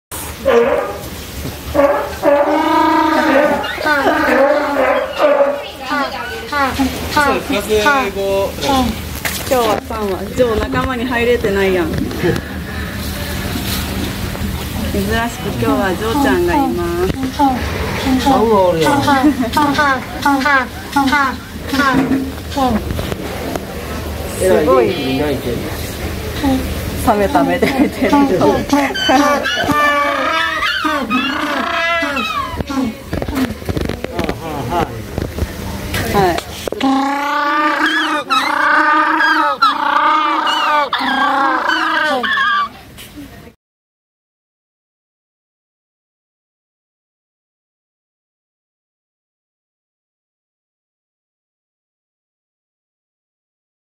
【ペンギン】ケープペンギンの鳴き声(^^)/ （伊勢シーパラダイス）
ケープペンギンの鳴き声を皆さんに紹介している短い動画です！
ペンギンの種類によっては多少鳴き方と言いますか、音の高低や音の伸ばし方が違いますがザクっと大きくくくるとだいたい似た様な鳴き方をしています(*´з`)

■各リンク
🔵伊勢シーパラダイスチャンネル